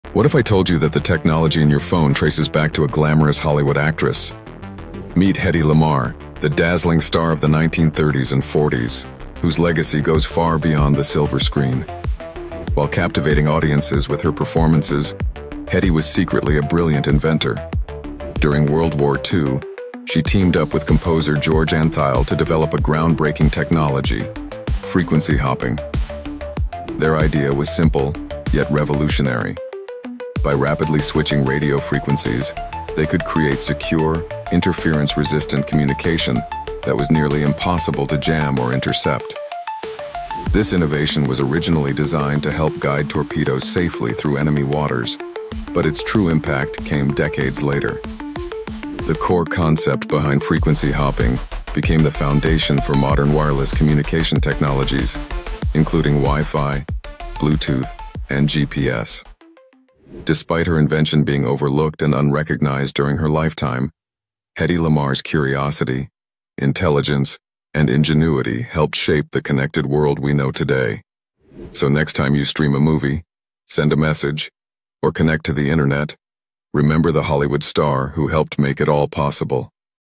Hedy Lamarr: The Star Who Changed Technology
Did you know a Hollywood icon helped invent Wi-Fi? Discover Hedy Lamarr’s secret life beyond the silver screen.